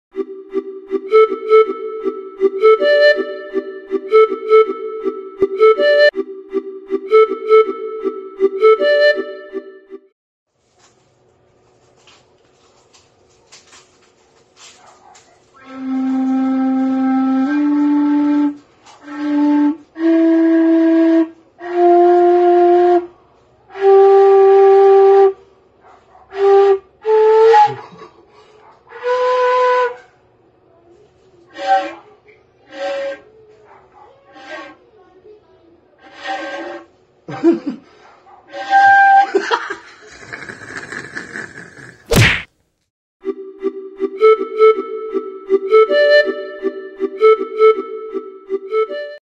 Homemade Bamboo Flute - Beginner's First Try (Just For Fun🤣🤣🤣)
This is my first homemade bamboo flute from the wild bamboos of Palawan, set in the key of C.

STAY IN TOUCH w/ AGAT